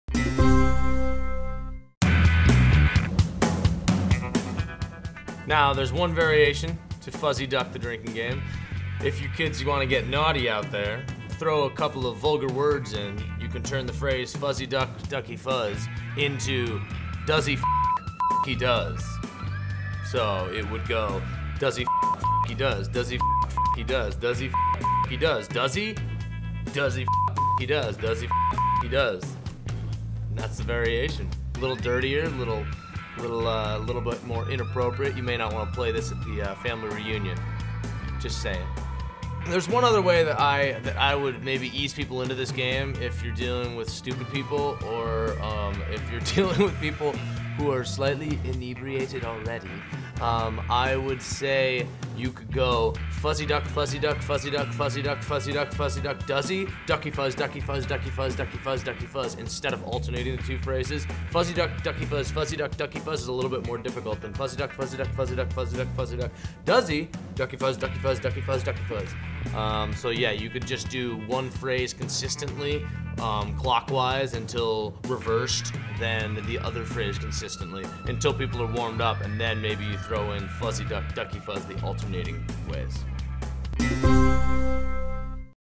How to Play Variations of Fuzzy Duck | Drinking Games
[MUSIC] Now, there's one variation to Fuzzy Duck, the drinking game. If you kids, you want to get naughty out there, throw a couple of vulgar words in. You can turn the phrase Fuzzy Duck or Ducky Fuzz into Duzzy [bleeped] or [bleeped]y Duzz. So, it would go Duzzy [bleeped] or [bleeped]y Duzz, Duzzy [bleeped] or [bleeped]y Duzz, Duzzy [bleeped] or [bleeped]y Duzz, Duzzy?, Duzzy [bleeped] or [bleeped]y Duzz, Duzzy [bleeped] or [bleeped]y Duzz. That's the variation. A little dirtier, a little bit more inappropriate. You may not want to play this at the family reunion. Just saying. There's one other way that I would maybe ease people into this game, if you're dealing with stupid people or if you're dealing with people who are slightly inebriated already. I would say you could go Fuzzy Duck, Fuzzy Duck, Fuzzy Duck, Fuzzy Duck, Fuzzy Duck, Fuzzy Duck, Duzzy?, Ducky Fuzz, Ducky Fuzz, Ducky Fuzz, Ducky Fuzz, Ducky Fuzz, instead of alternating the two phrases. Fuzzy Duck, Ducky Fuzz, Fuzzy Duck, Ducky Fuzz is a little bit more difficult than Fuzzy Duck, Fuzzy Duck, Fuzzy Duck, Fuzzy Duck, Fuzzy Duck, Duzzy?, Ducky Fuzz, Ducky Fuzz, Ducky Fuzz, Ducky Fuzz. So, yeah, you could just do one phrase consistently clockwise until reversed then the other phrase consistently until people are warmed up and then, maybe, then you throw in Fuzzy Duck, Ducky Fuzz, the alternating ways.